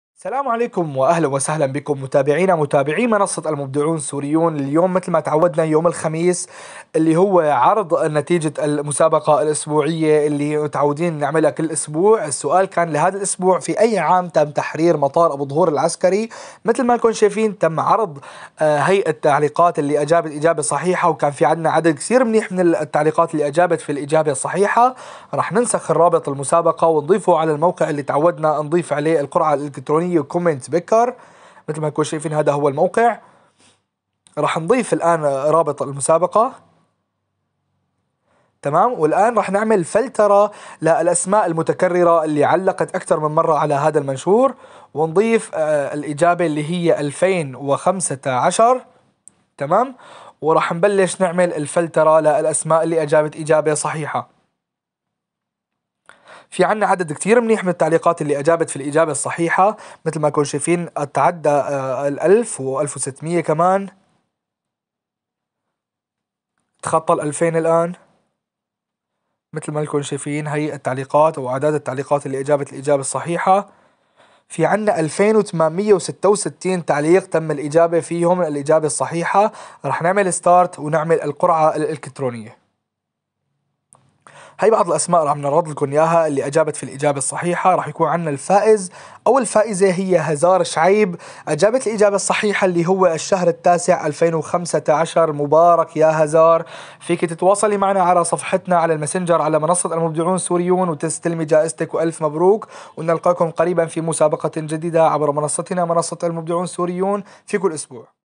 الرابح معنا في مسابقة_الأسبوع : (هزار شعيب)
السؤال : في أي عام تم تحرير مطار أبو الظهور العسكري؟
الإجابة : 2015